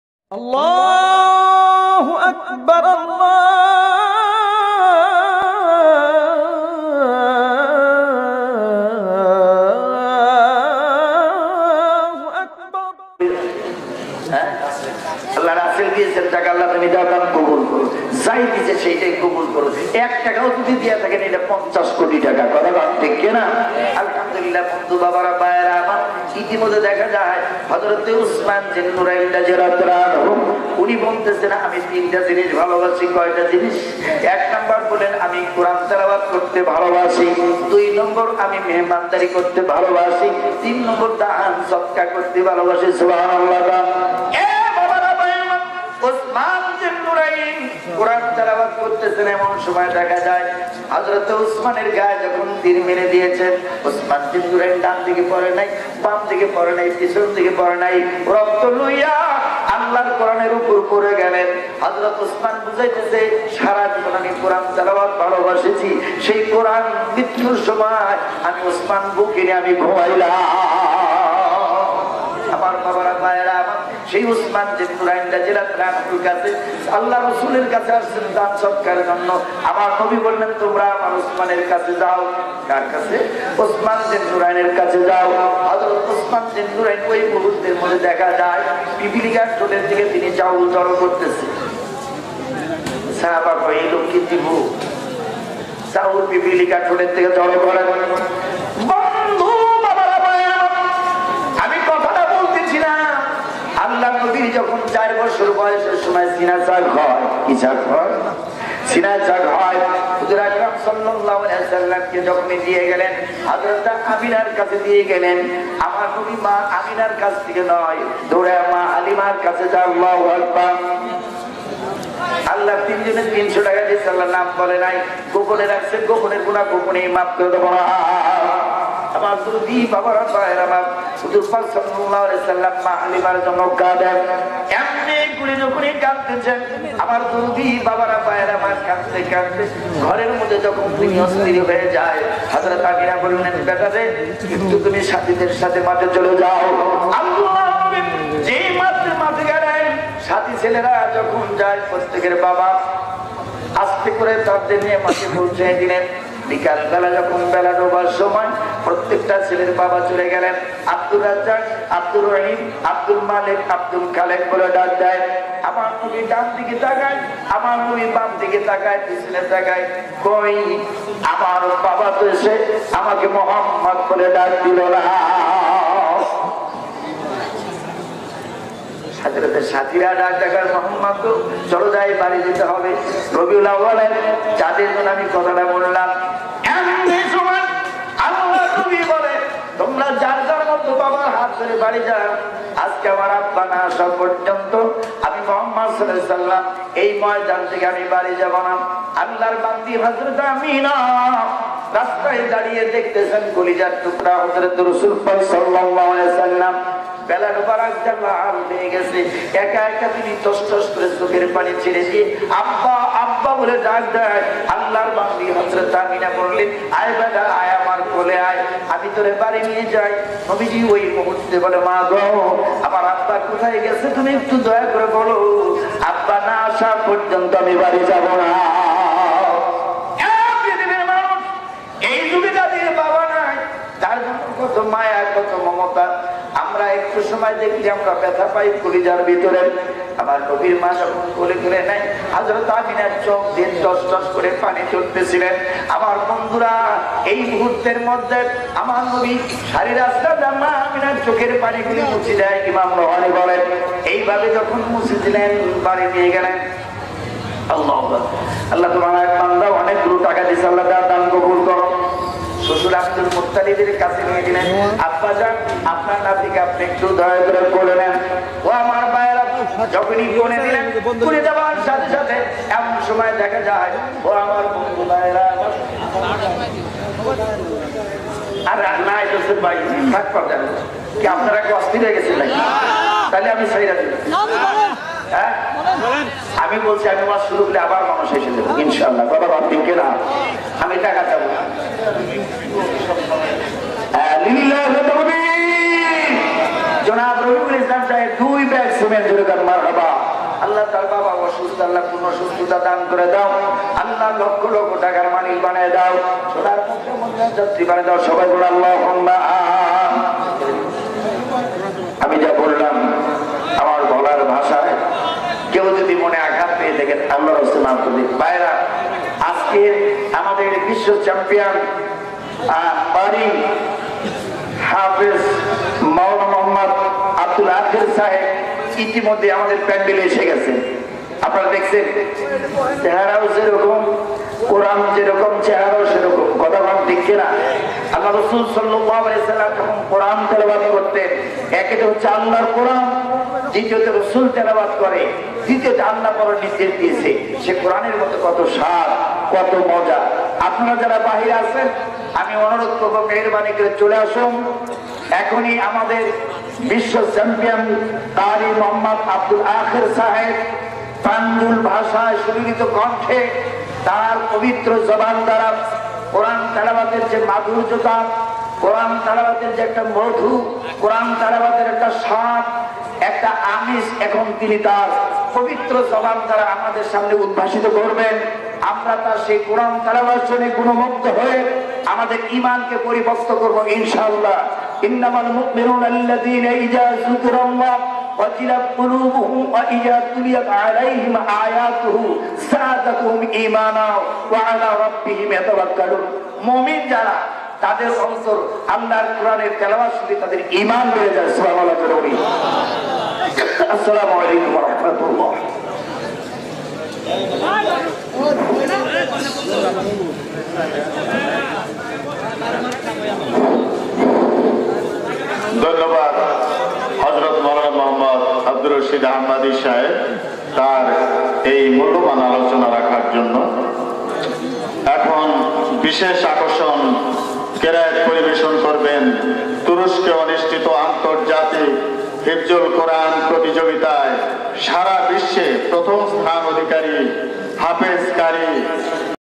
দান-সদকা সম্পর্কে বয়ান মাওলানা আব্দুর রশিদ সাহেব,টাঙ্গাইল।
AL-Madina.Tv-Tangail
mawlana abdur rashid, Tangail.